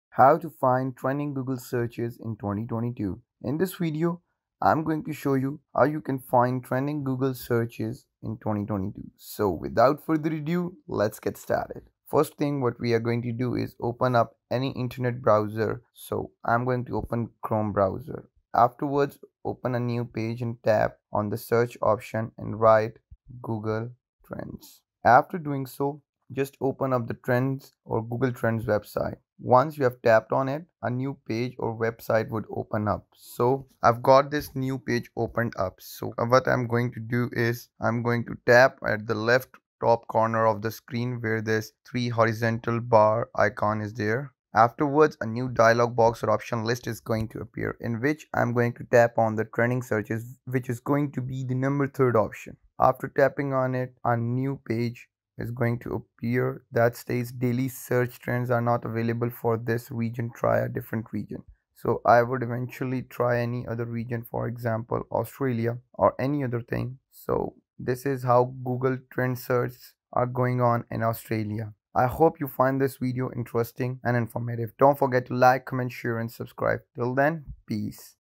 How To Find Trending Google Searches | Easy Tutorial (2025)
How To Find Trending Google Searches (2025)

Learn How To Find Trending Google Searches. It is super easy and anyone can do it in just a few minutes by following these simple steps.